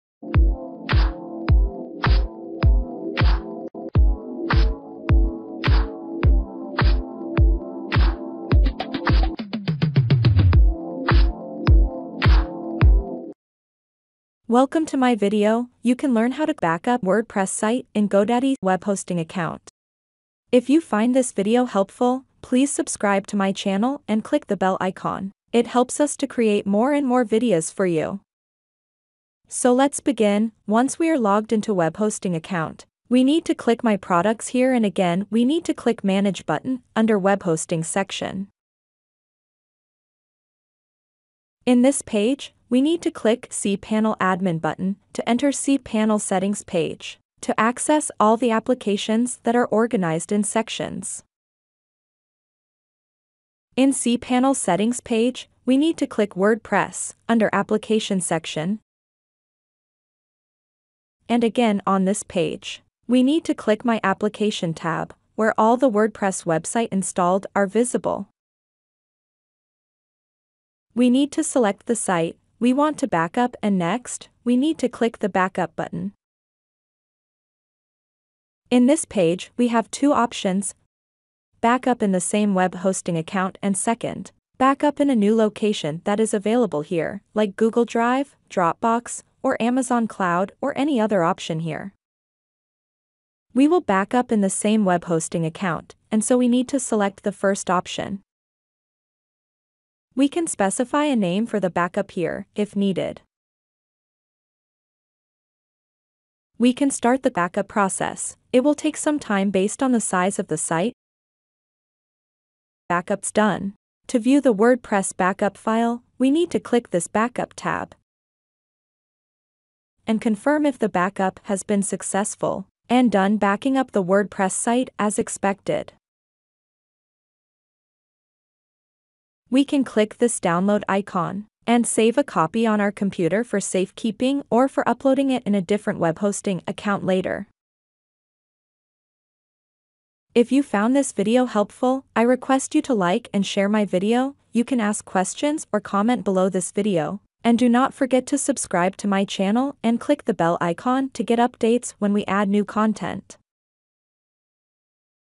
How can you backup your wordpress site in a under 3 minutes 2022
Backup Wordpress Site in Godaddy Webhosting Account